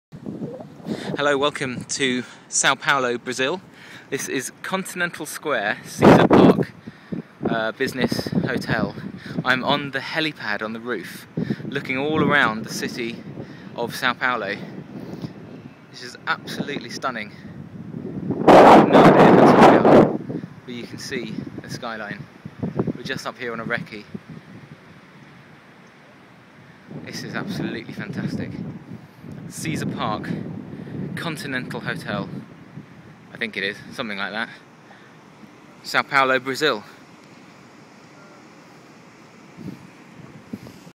Sao Paulo from Helipad on Caesar Business building
Skyline view of Sao Paulo city, Brazil, from helipad on Caesar Business building